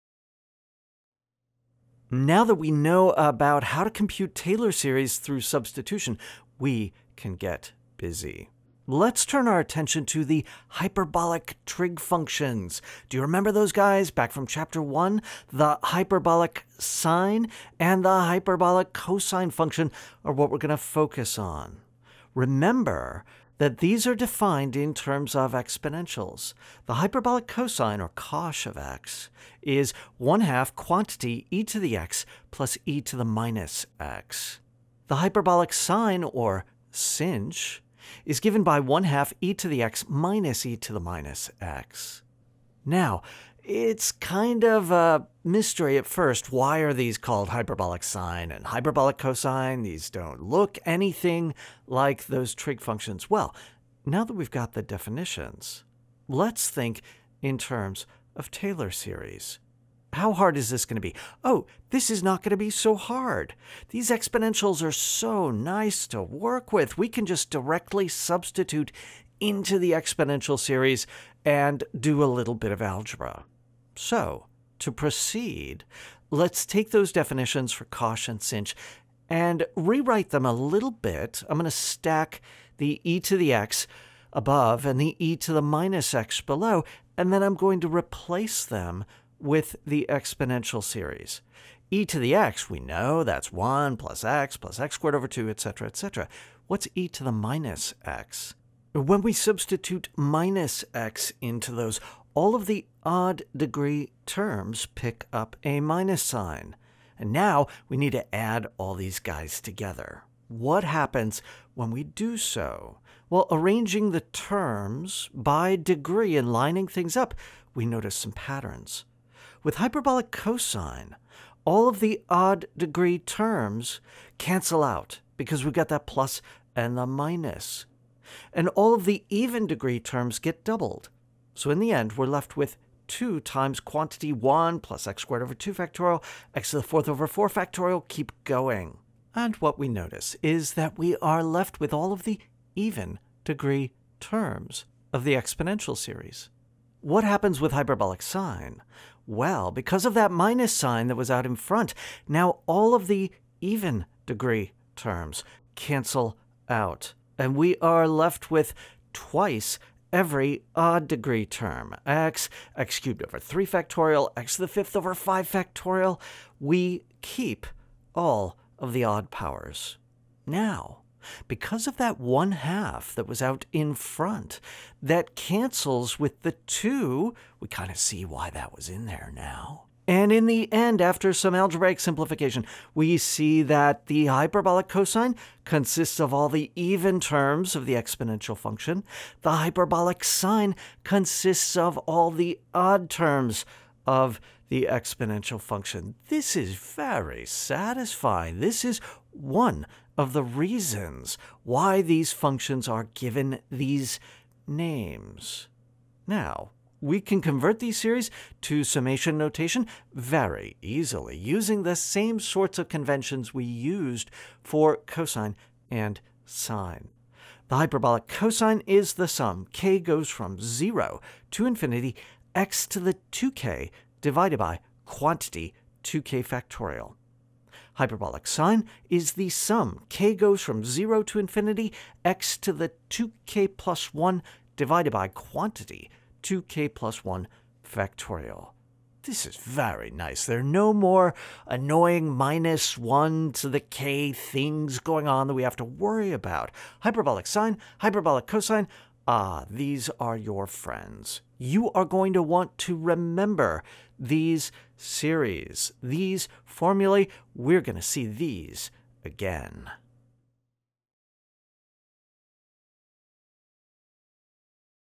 CalcGREEN 1 : Ch. 4.3 : Series for SINH & COSH
Now that we know how to compute Taylor series easily through substitution, let's revisit the hyperbolic trig functions from Chapter 1. As we'll see, the Taylor series gives a hint as to why these function are called hyperbolic sine & cosine...